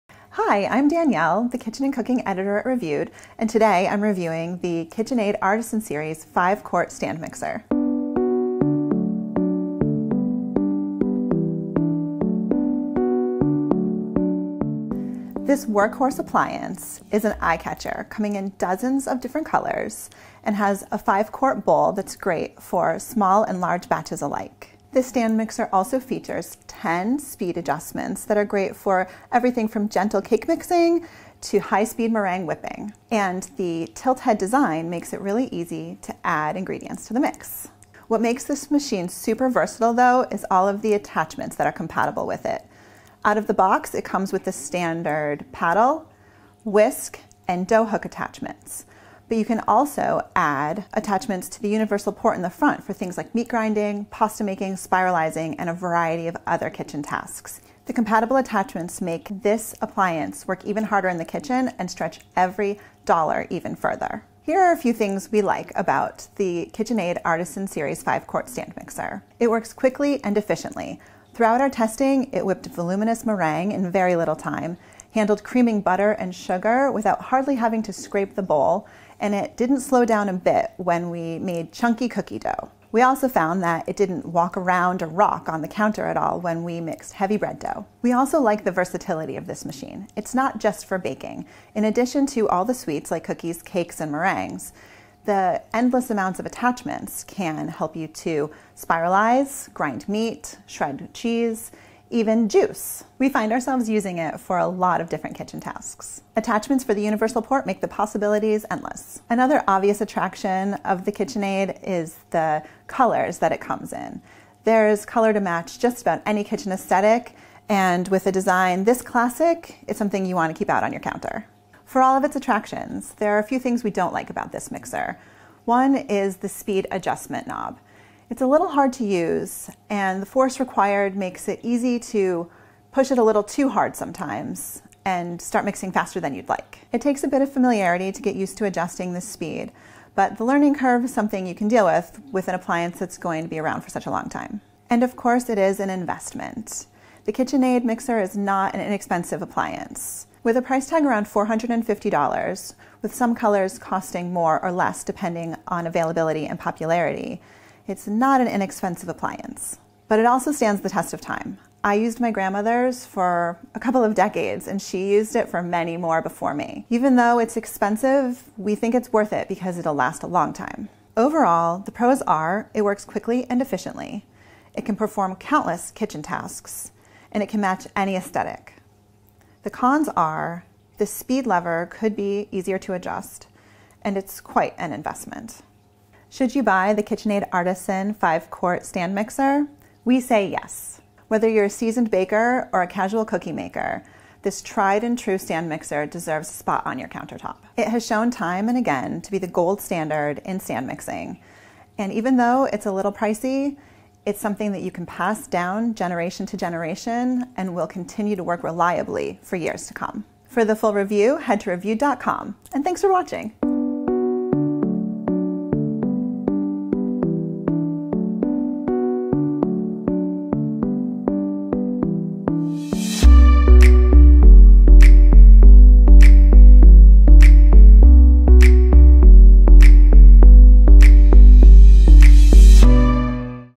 KitchenAid Artisan Series Stand Mixer Review: The Ultimate Kitchen Companion
00:00 - Meet our Kitchen & Cooking Editor
00:15 - KitchenAid Artisan Series
00:38 - KitchenAid Attachments
01:09 - Quick & Efficient
01:28 - Versatile & Multifunctional
01:49 - Fits any aesthetic
02:01 - Cons: Speed adjustment knob
02:25 - Cons: Expensive
02:51 - Overall: Pros / Cons
03:06 - Should you buy the KitchenAid Artisan Series Stand Mixer?
03:30 - Conclusion

Whether you're a seasoned baker or casual cookie maker, the KitchenAid Artisan Series 5-Quart Stand Mixer deserves a space on your countertop.

It's shown us—time and time again—that it blows its competitors out of the water with its fast and efficient results for all kinds of kitchen tasks. Beyond impressively whipping egg whites and kneading homemade bread dough, this machine can come in handy for everyday kitchen tasks (thanks to its countless accessories). It can ultimately save you precious time—and may even help you feel more confident in the kitchen.

— Recommendations are independently chosen by Reviewed’s editors. Purchases you make through our links may earn us a commission.